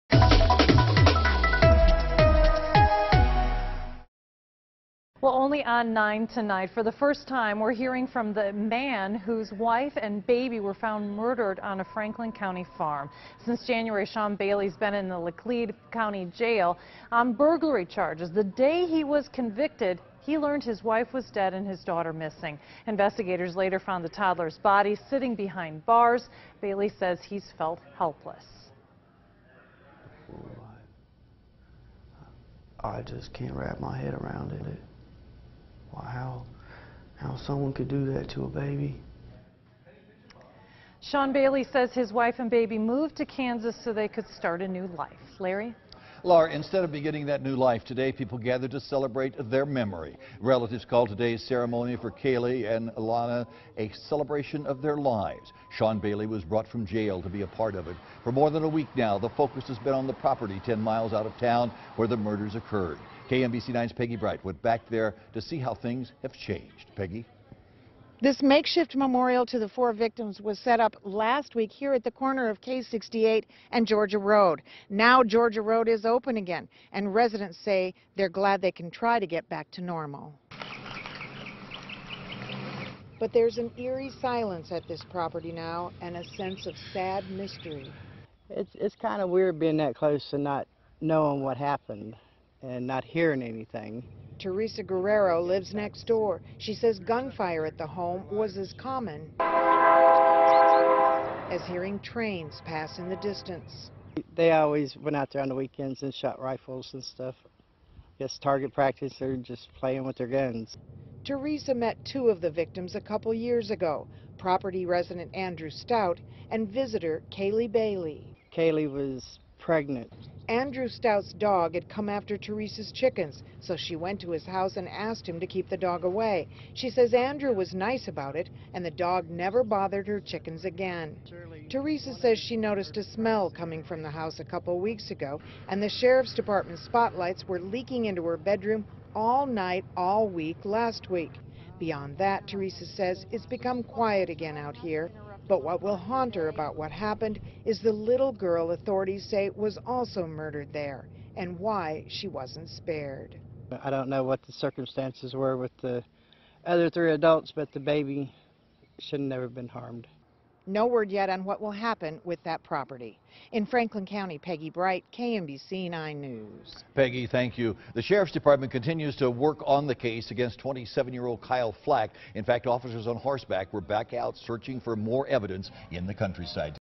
Man discusses farm slayings of daughter, estranged wife
The estranged husband of Kaylie Bailey and the father of slain toddler Lana Leigh Bailey talks about losing both of them. He is being held in a Missouri jail on an unrelated charge.